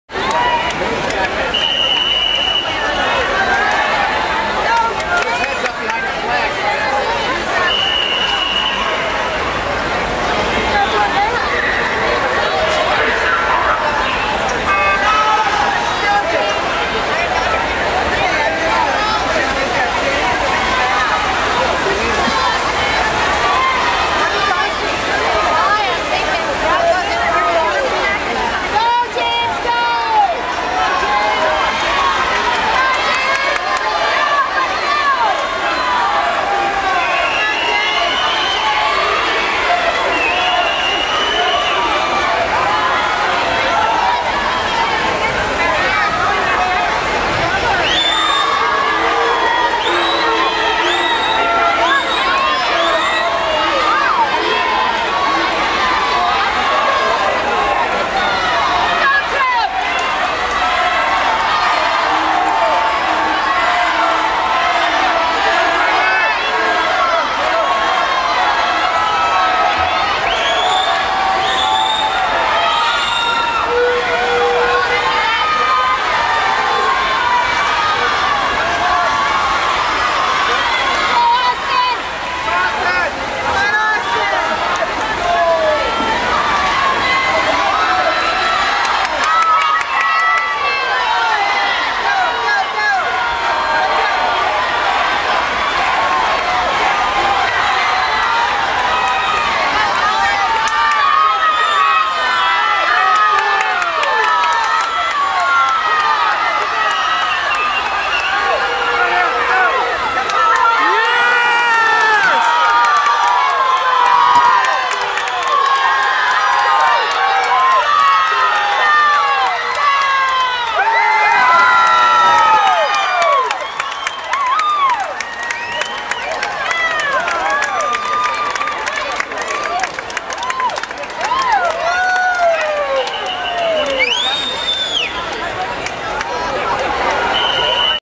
Never Quit
YMCA State Medley Relay Final: come from behind win in lane 4.  No matter how far behind you might get, you never give up!